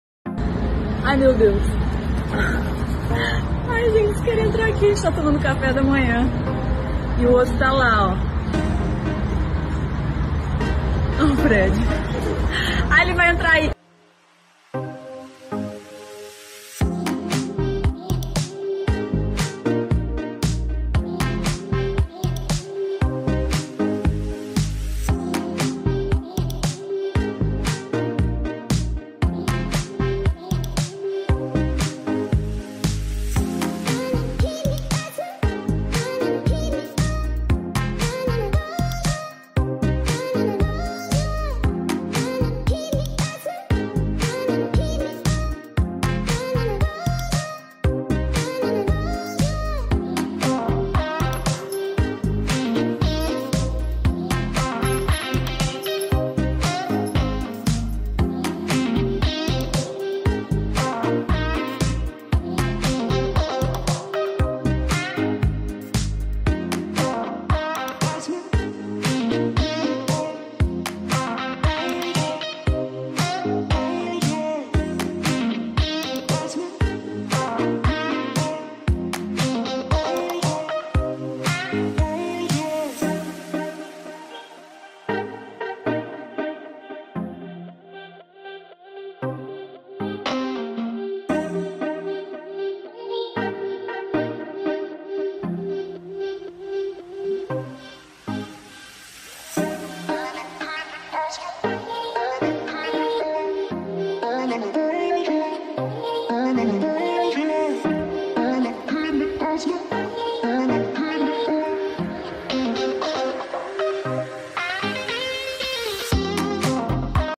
Toucans visiting an apartment
Hello everyone, this is your daily dose of Cuteness. In this video there's toucans visiting an apartment!